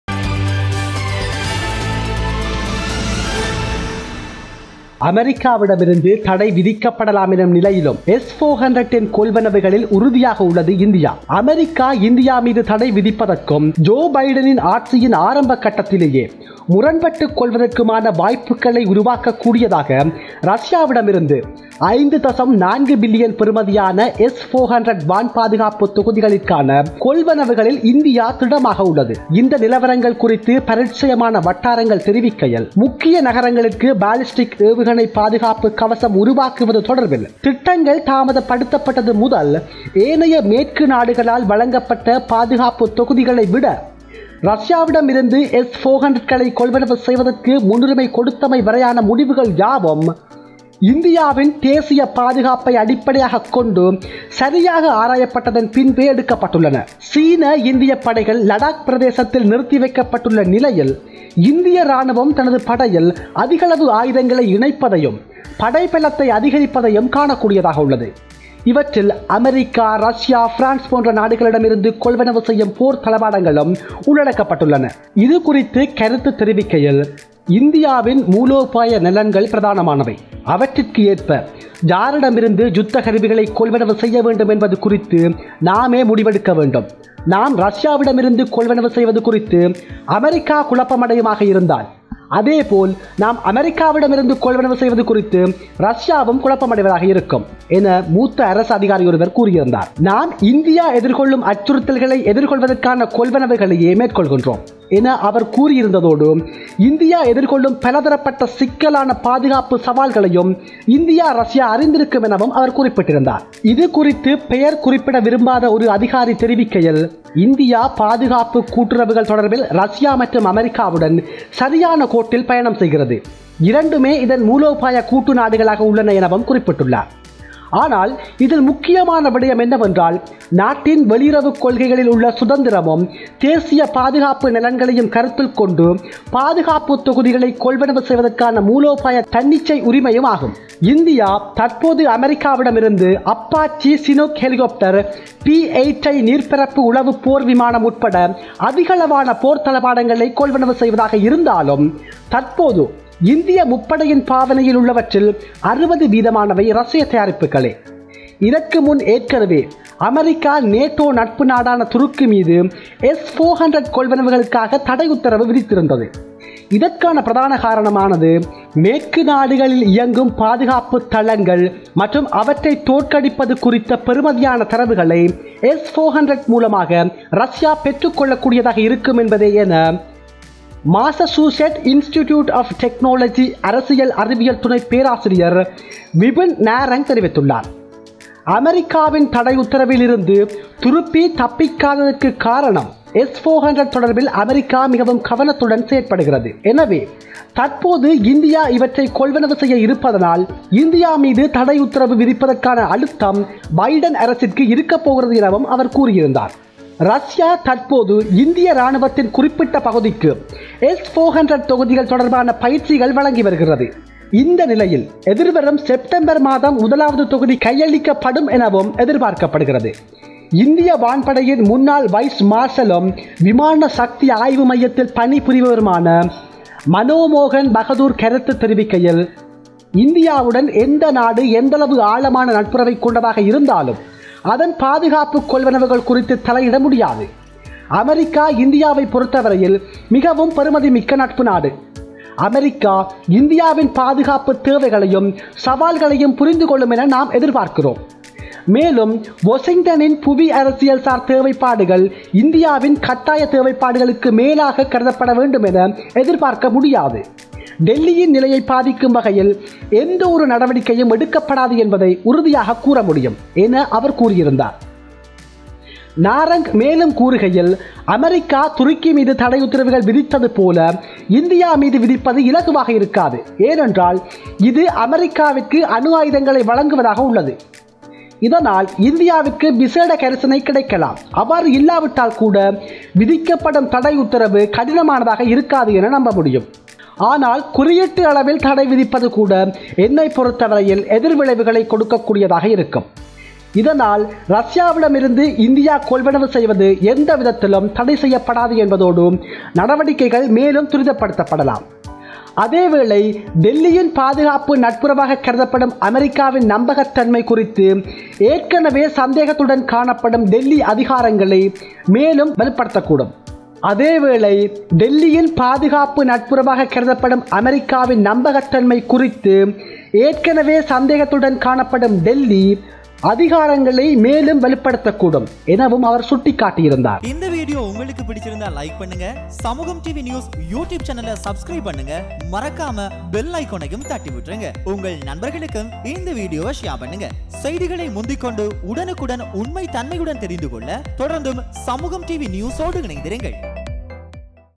India remains steadfast in its purchases of the S-400 despite the possibility of ban from the US.
The United States has offered to buy $ 5.4 billion worth of s-400 air defense blocks from Russia, which could create opportunities for sanctions against India and conflict in the early stages of Joe Biden's rule.India is solid in purchases.